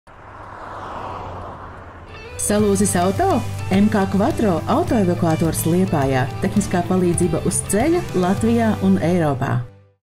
Autoevakuācija un tehniskā palīdzība uz ceļa Latvijā un Eiropā - M.K. Quattro
M.K. Quattro autoevakuators Liepājā - tehniskā palīdzība uz ceļa un auto evakuācija visā Latvijā un Eiropā.
M.K. Quattro varēs evakuēt dažāda veida transportlīdzekļus - vieglo auto evakuācija, mikroautobusu evakuācija, motociklu evakuācija.